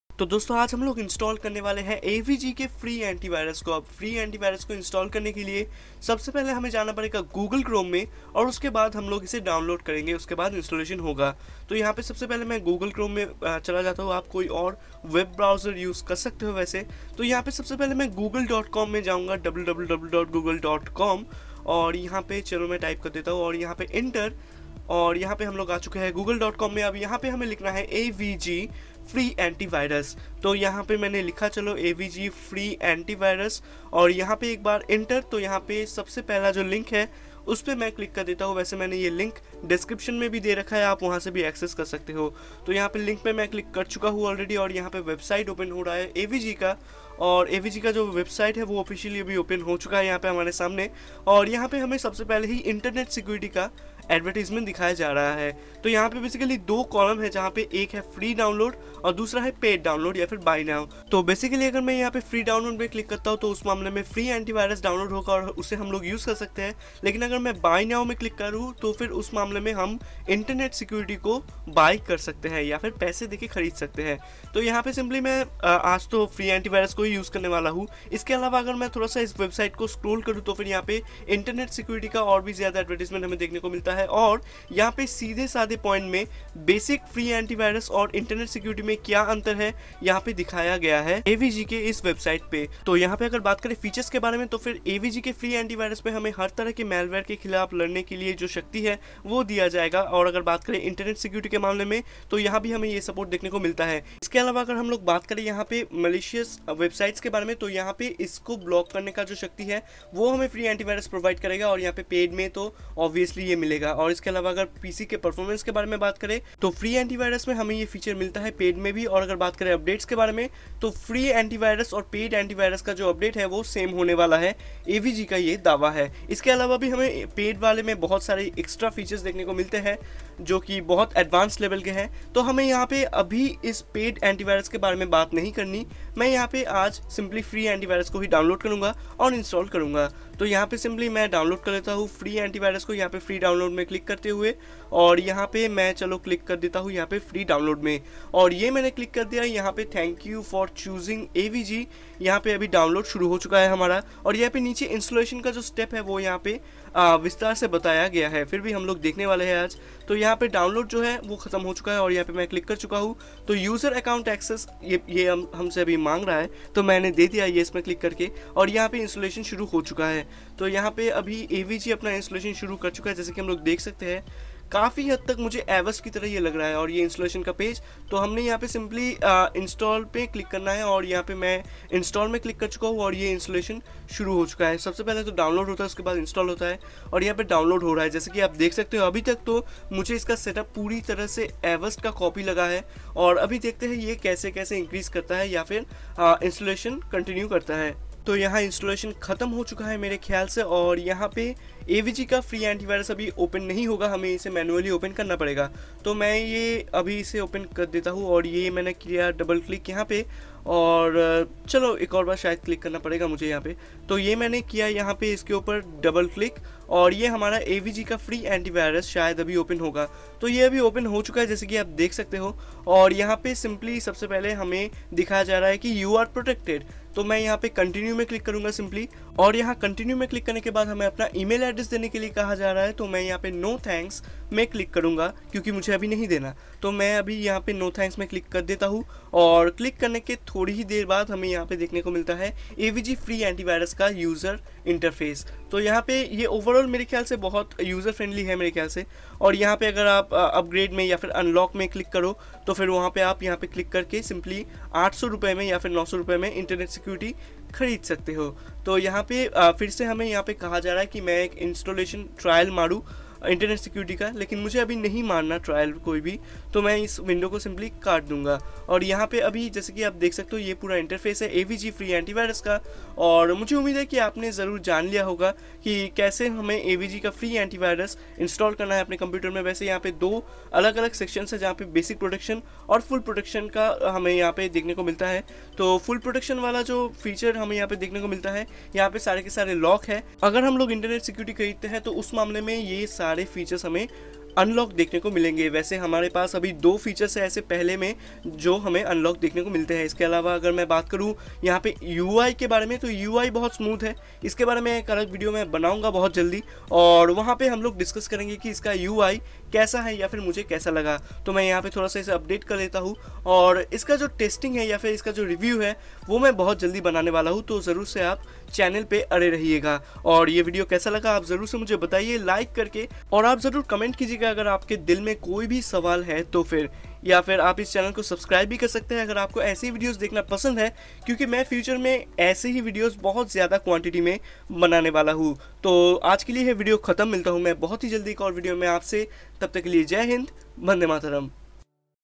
AVG Free Antivirus (2018)!!! Full Installation Explained In Hindi!!!
Namashkar Dosto kaise ho app aj iss video mein hum baat karene wale AVG free antivirus k barey mein aur hum janengey ki kaise hum avg free antivirus ko free mein iski hi official website se download kar sakte hai aur bindas use kar sakte hai toh aiyee suru karte hai...